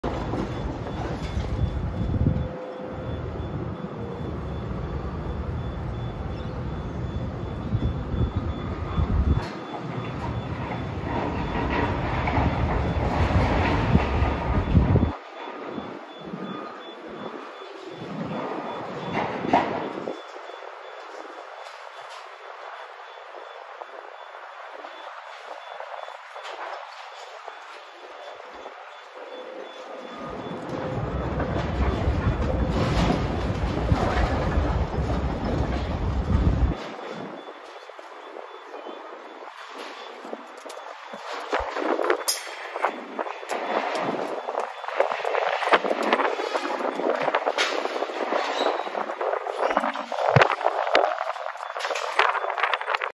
Darling harbour monorail station demolished
Many years ago in 1988 the Sydney monorail was there from almost 35 years ago and then in 2013 which is 10 years ago the Sydney monorail is now gone and some the station is gone